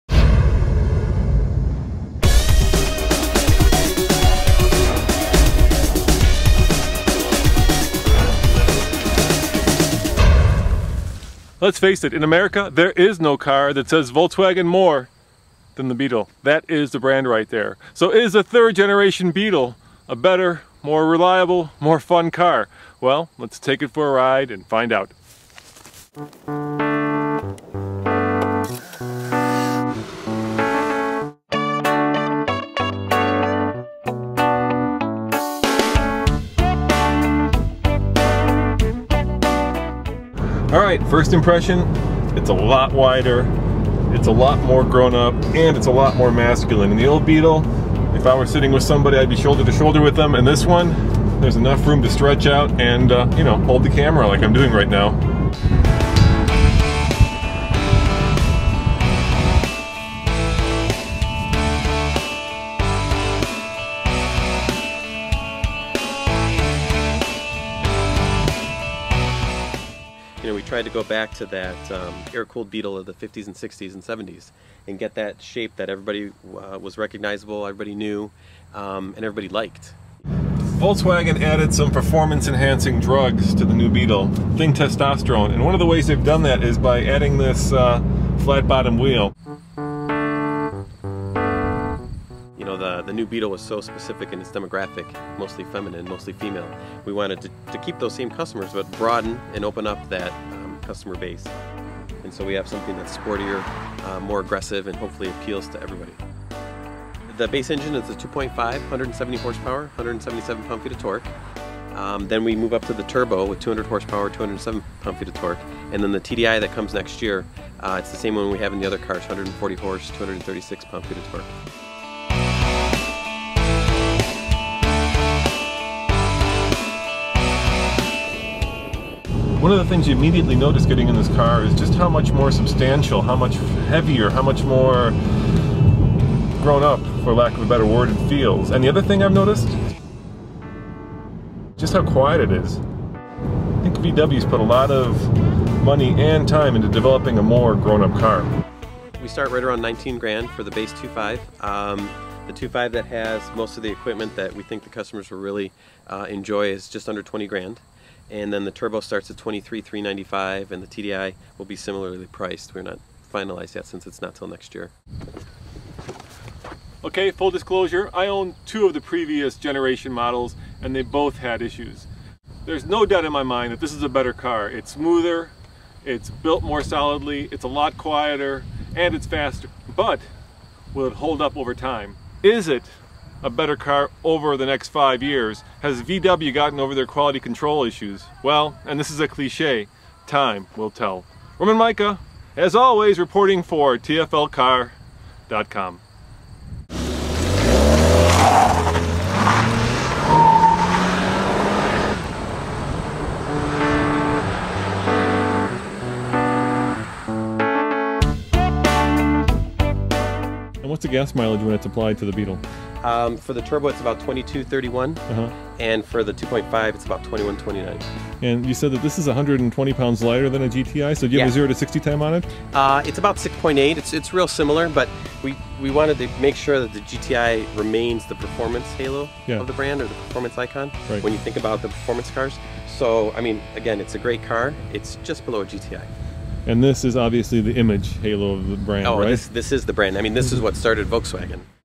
2012 Volkswagen Beetle Turbo First Drive Review
) The 2012 Volkswagen Beetle Turbo is about 120 pounds lighter than the 2012 VW Golf GTI. Both VW cars share the same 2.0L turbo engine. So is the 2012 Beetle turbo faster? Check out this first drive review as we test the new 2012 Beetle in the rolling hills of Virginia.